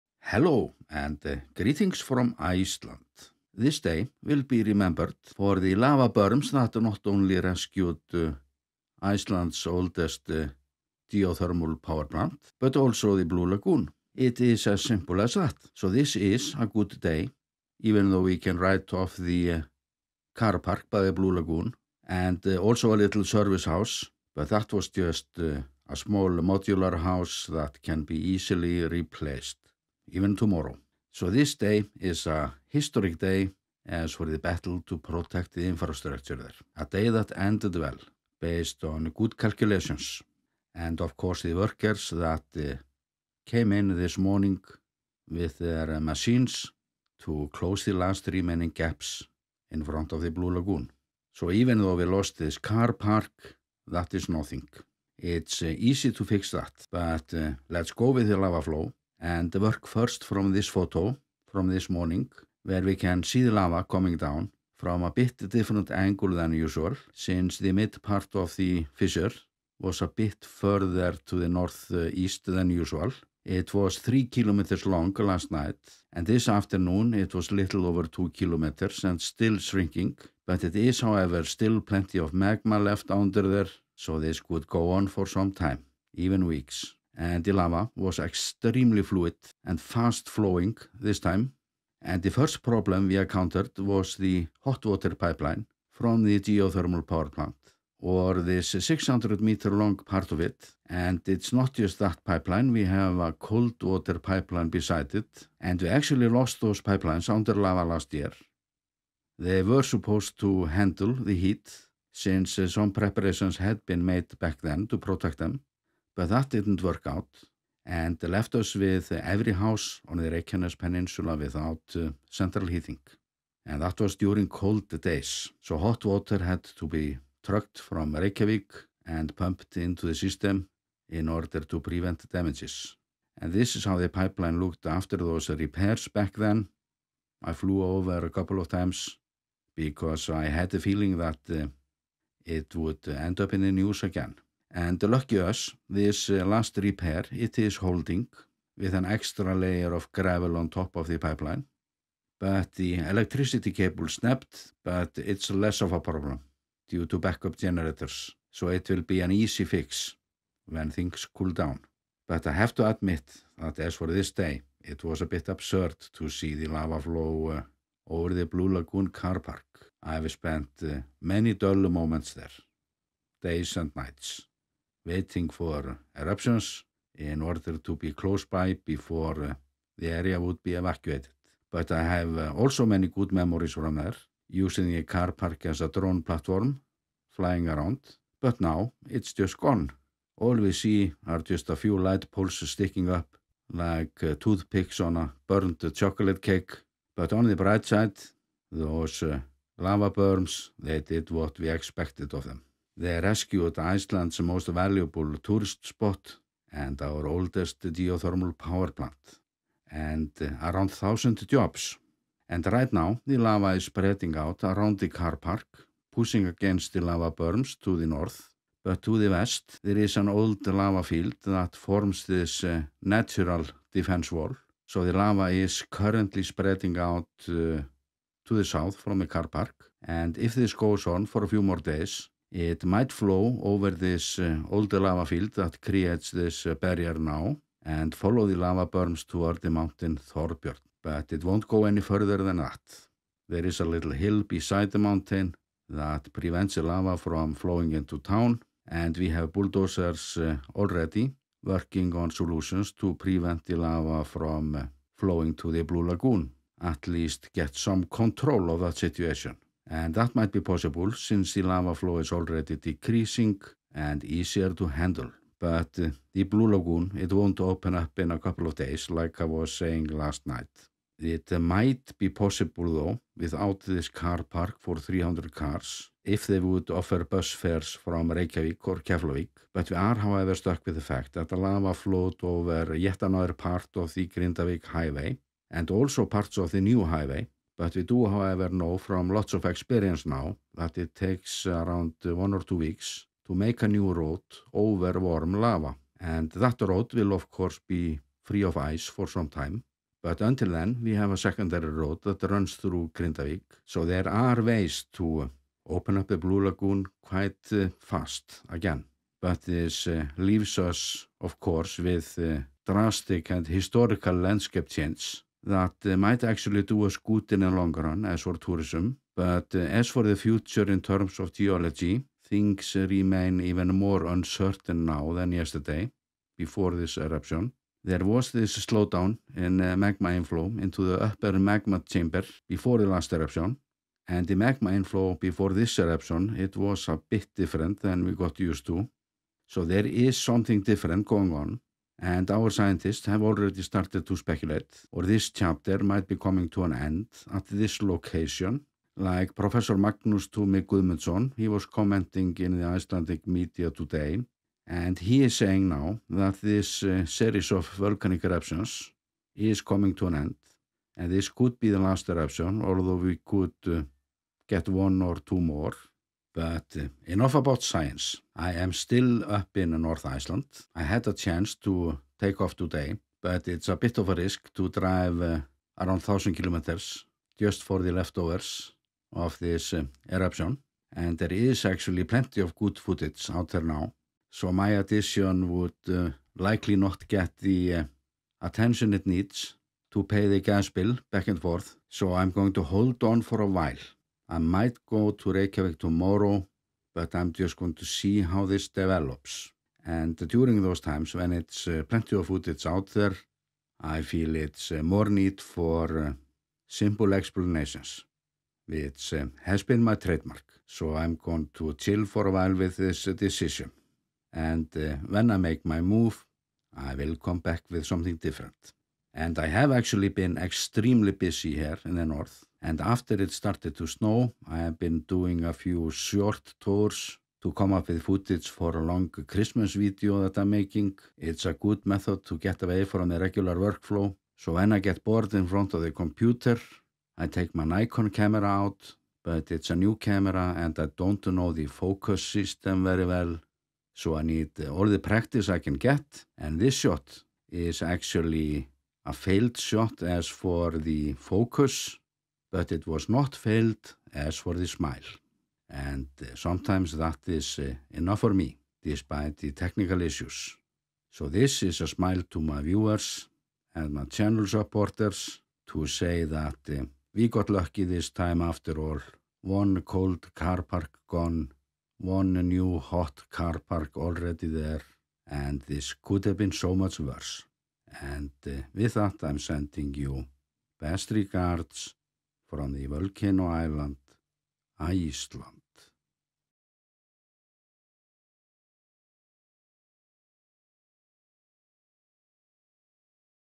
The Blue Lagoon Incident Was Only Innovative Engineering Saving Valuable Infrastructure
The Blue Lagoon car-park incident  todaywas only a clickbait disaster when we look at the enormous value our lava berms just rescued. Iceland's oldest power plant, Icelands hottest tourists spot, several hotels and over 1000 jobs.  All saved my  truckloads of gravel! So it was a bit frustrating for us who understand this scenario to look at all the clickbait fuzz over a carpark. So here is my take on this because this is in fact a good day for us Icelanders,   Innovative engineering at its best and this chapter in our history might be over and done with in the nest months.